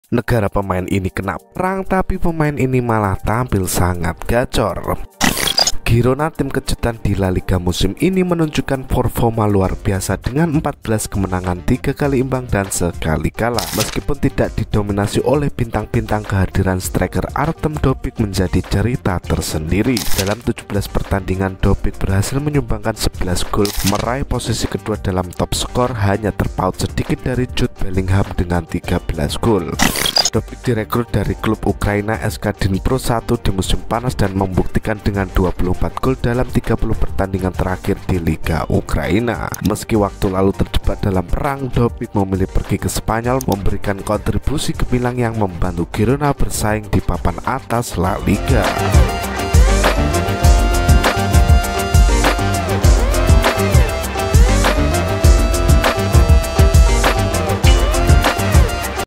di saat negaranya berperang artem dovbyk malah bermain sangat baik
SiPaling Network
SiPaling Network Bola